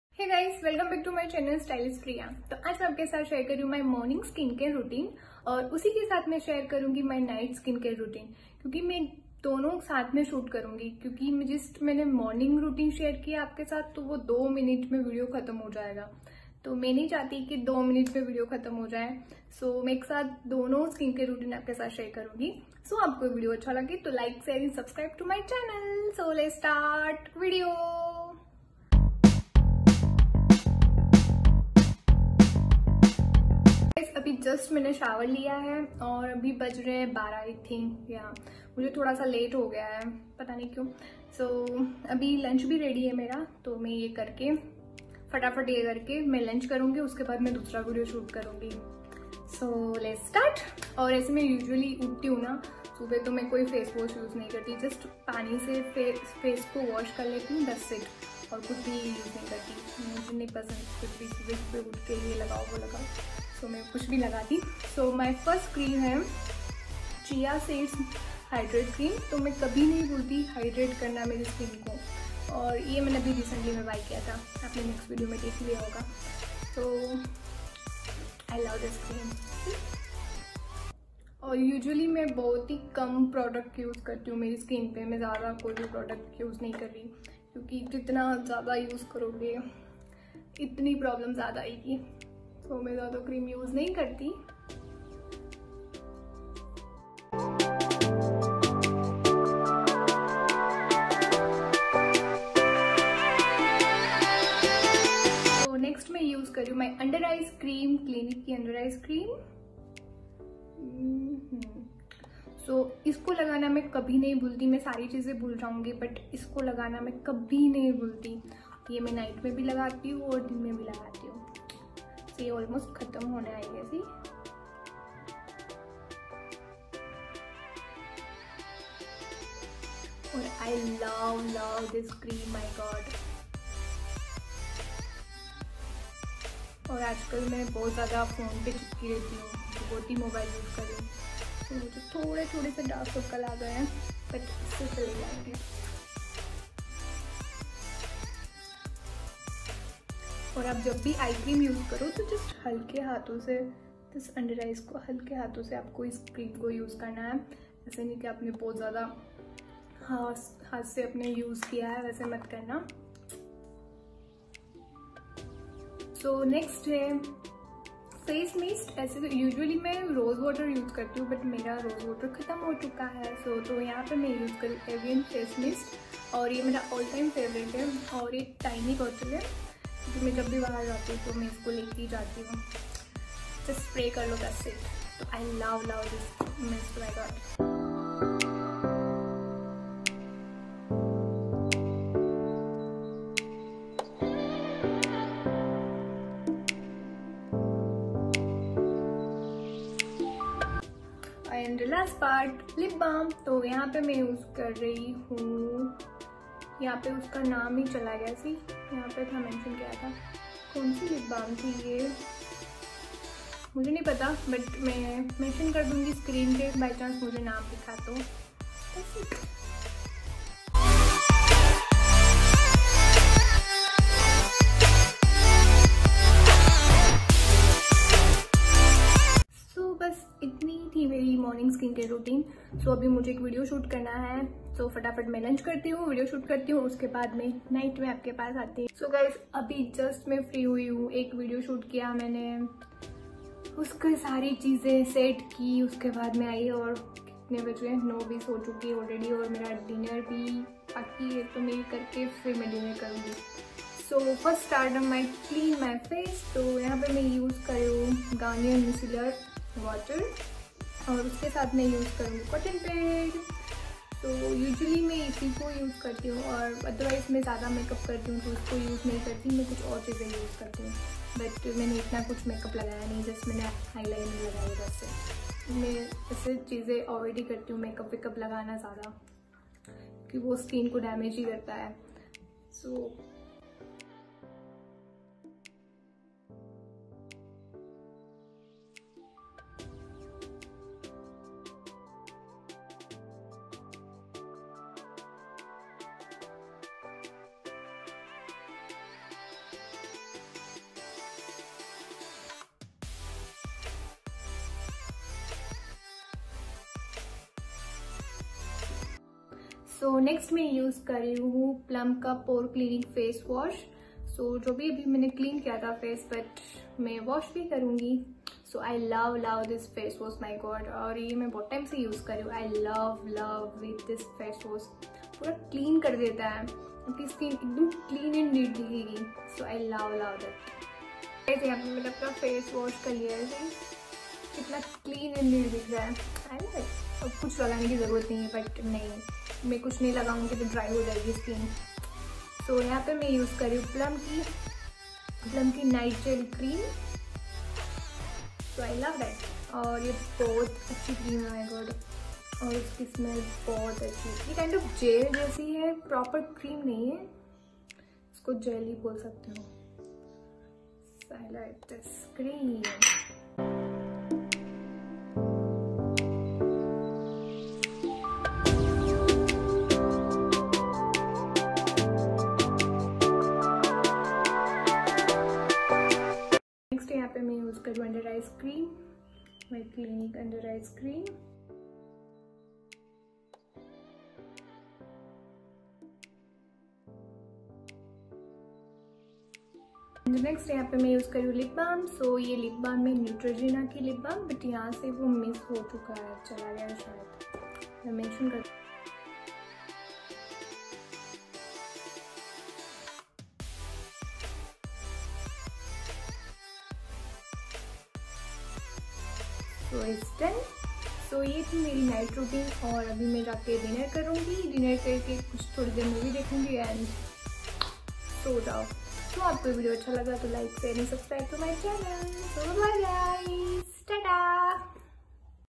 My Daily life Skincare Routine | Morning & Night Routine....
Easy skin care routine
Morning and night skin care routine in easy and quick skin care for flawless skin...

xoxo